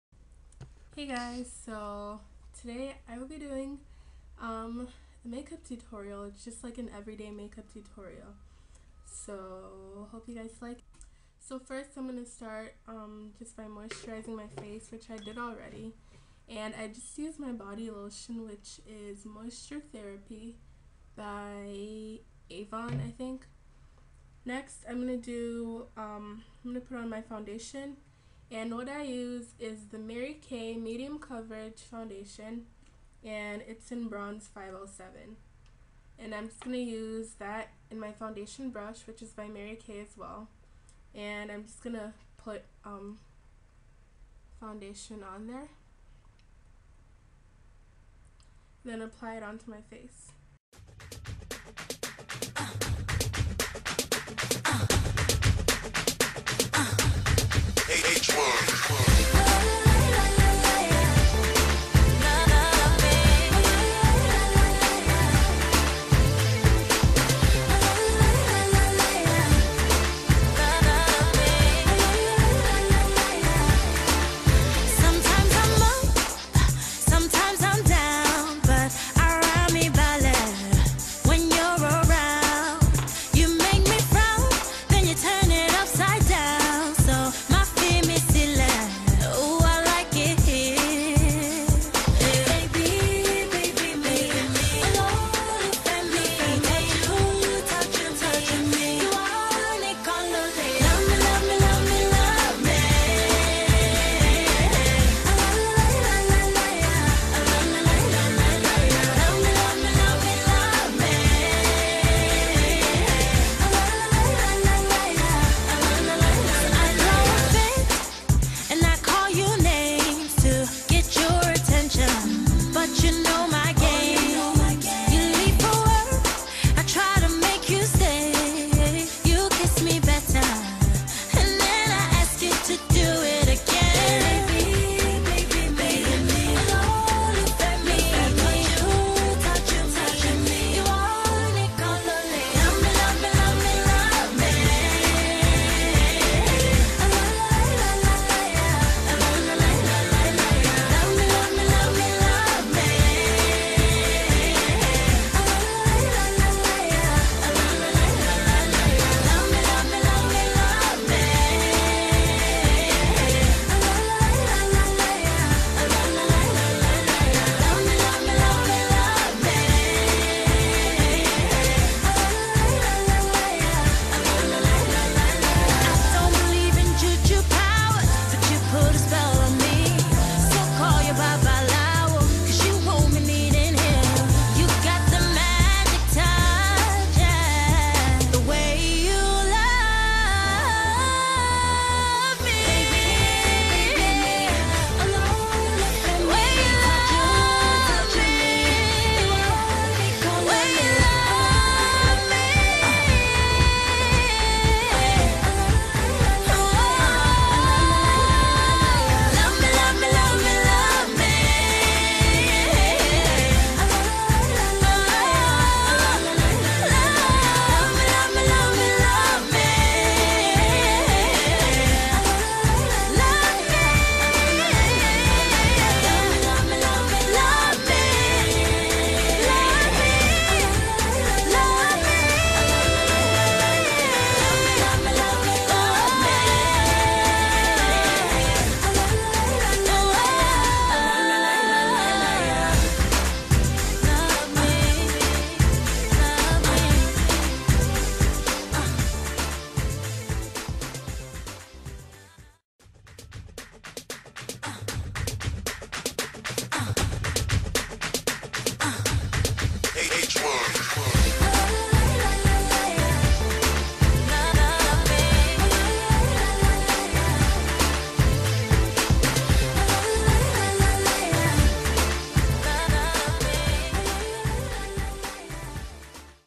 Everyday Makeup Tutorial :)
My everyday makeup routine(well, whenever I do wear makeup.lol) Hope you fellow youtubers like :)

Products used:

Mary Kay Medium Coverage Foundation(Bronze 507)

L'oreal True Match Powder

Mary Kay Blush

Mary Kay Eyeshadow

Mary Kay Lengthening Mascara

Maybelline Eye Studio Gel Eyeliner

NYX White Eyeliner

Mary Kay Nourishine Lipgloss

Avon Sparkle Lip Gloss